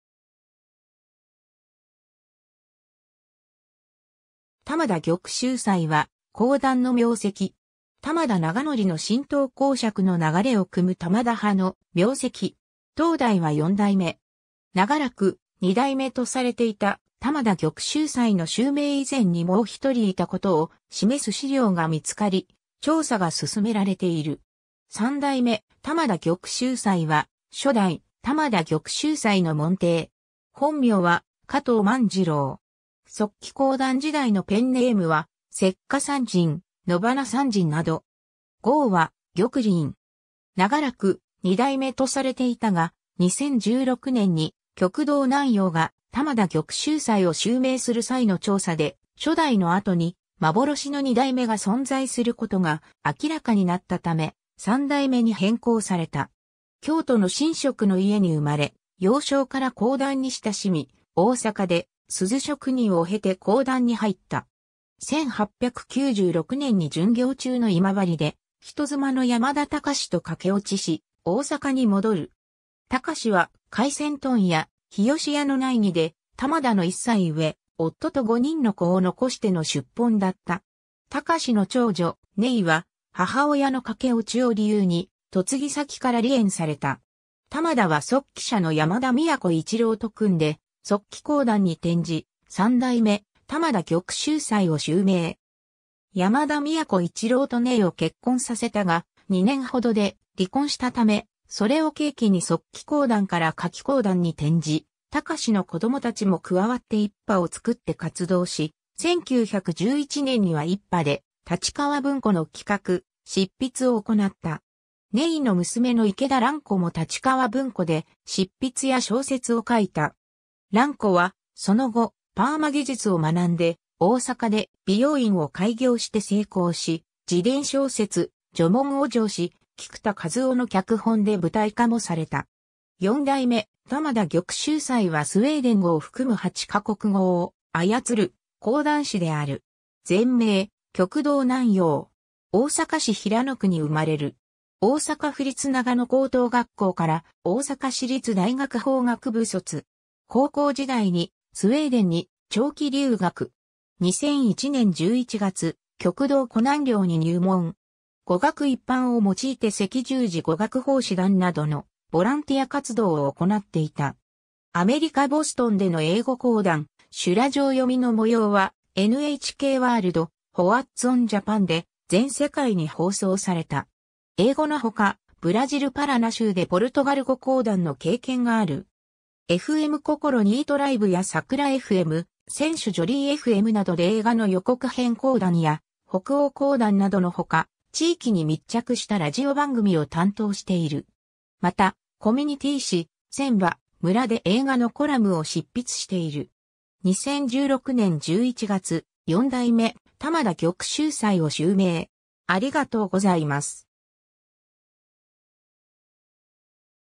玉田玉秀斎
玉田 玉秀斎（たまだ ぎょくしゅうさい）は、講談の名跡。
玉田永教の神道講釈の流れをくむ玉田派の名跡。
当代は四代目。
 長らく「二代目」とされていた玉田玉秀斎（下記）の襲名以前にもう一人いたことを示す資料が見つかり、調査が進められている。
 三代目玉田 玉秀斎（1856年- 1919年）は初代玉田玉秀斎の門弟。
本名は加藤 万次郎。
速記講談時代のペンネームは「雪花山人」、「野花散人」など。
号は「玉麟」。
長らく「二代目」とされていたが、2016年に旭堂南陽が玉田玉秀斎（現・四代目）を襲名する際の調査で、初代のあとに「幻の二代目」が存在することが明らかになった為、三代目に変更された。
 京都の神職の家に生まれ、幼少から講談に親しみ、大阪で錫職人を経て講談に入った。
1896年に巡業中の今治で人妻の山田敬と駆け落ちし大阪に戻る。
敬は廻船問屋「日吉屋」の内儀で玉田の一歳上、夫と5人の子を残しての出奔だった。
敬の長女・寧は母親の駆け落ちを理由に嫁ぎ先から離縁された。
玉田は速記者の山田都一郎と組んで速記講談に転じ、3代目玉田玉秀斎を襲名。
山田都一郎と寧を結婚させたが、2年ほどで離婚したため、それを契機に速記講談から書き講談に転じ、敬の子供たち（長女、長男、三男、四男）も加わって一派を作って活動し、1911年には一派で立川文庫の企画・執筆を行なった。
寧の娘の池田蘭子（1893年－1976年）も立川文庫で執筆や小説を書いた。
蘭子はその後パーマ技術を学んで大阪で美容院を開業して成功し、自伝小説『女紋』を上梓、菊田一夫の脚本で舞台化もされた。
 四代目玉田 玉秀斎（1976年11月30日 - ）はスウェーデン語を含む8か国語を操る講談師である。
前名旭堂 南陽（きょくどう なんよう）。
 大阪市平野区に生まれる。
大阪府立長野高等学校から大阪市立大学法学部卒。
高校時代にスウェーデンに長期留学。
2001年11月、旭堂小南陵（のち4代目旭堂南陵）に入門。
 語学一般（英語・スウェーデン語・ドイツ語・中国語等）を用いて赤十字語学奉仕団などのボランティア活動を行っていた。
 アメリカ・ボストンでの英語講談・修羅場読みの模様はNHK WORLD「what's on Japan」で全世界に放送された。
 英語の他、ブラジル・パラナ州でポルトガル語講談の経験がある。
 FM COCOLO NETRIBE(76.5mhz)やさくらFM(78.7mhz)、SENSHU JOLLY FM(82.6mhz)などで映画の予告編講談や北欧講談などの他、地域に密着したラジオ番組を担当している。
 また、コミュニティー誌「せんば村」で映画のコラムを執筆している。
 2016年11月、四代目玉田玉秀斎を襲名。